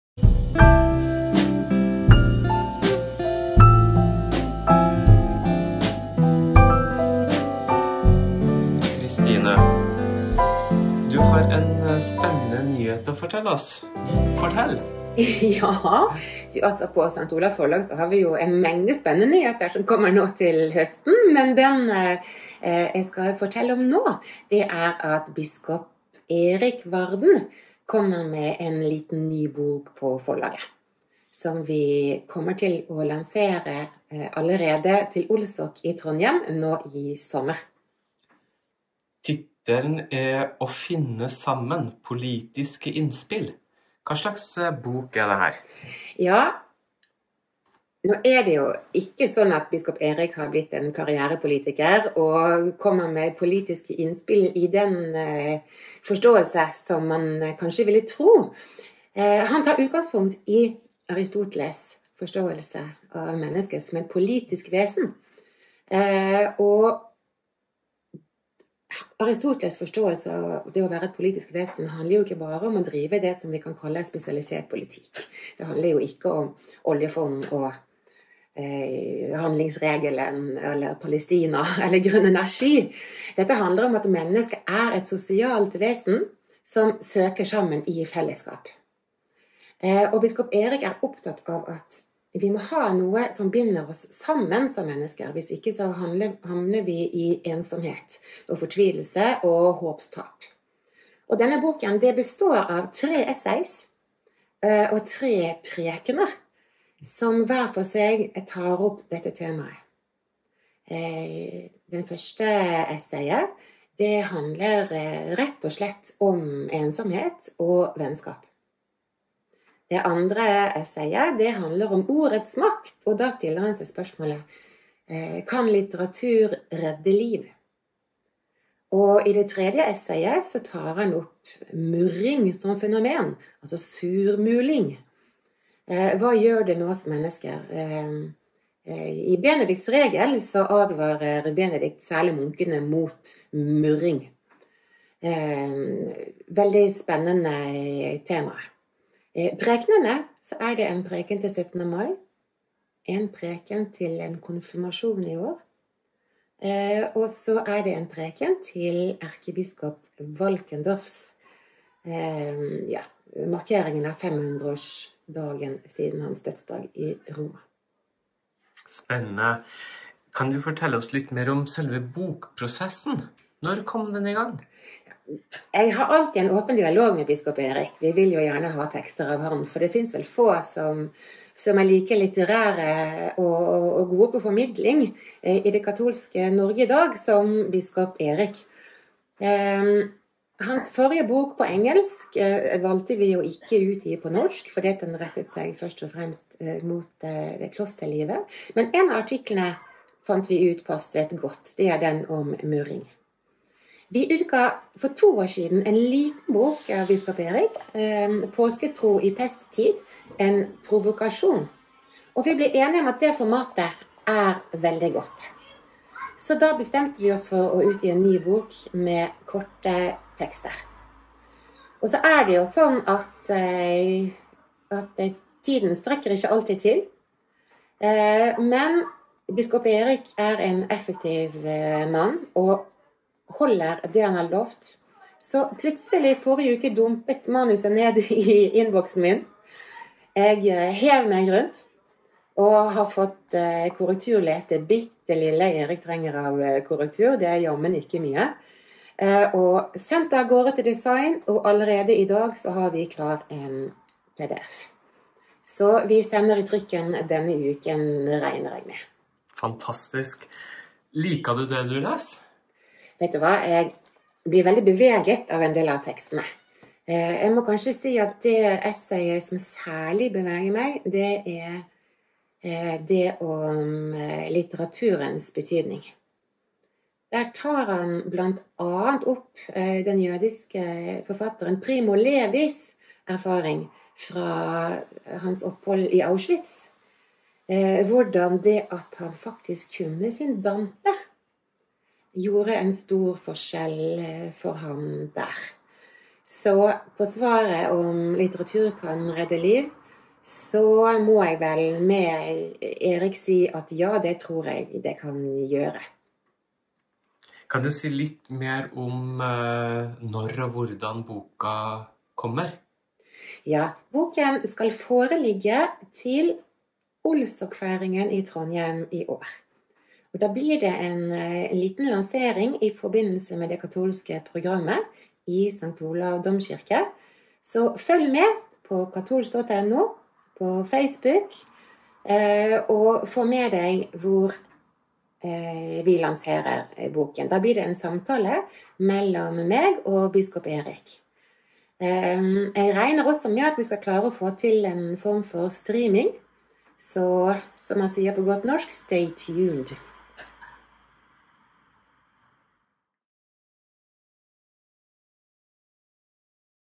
Biskop Erik Varden med ny bok
Under årets olsokfeiring lanserer biskop Erik Varden av Trondheim boken «Å finne sammen – politiske innspill».
– I denne boken tar Varden utgangspunkt i Aristoteles forståelse av mennesket som et politisk vesen. Det handler ikke bare om å drive spesialisert politikk, men om at mennesket er et sosialt vesen, som søker sammen i fellesskap. Biskop Erik er opptatt av at vi må ha noe som binder oss sammen, hvis ikke havner vi i ensomhet, forklarer Kristine Dingstad, forlagssjef i Oslo katolske bispedømmes forslag St. Olav forlag.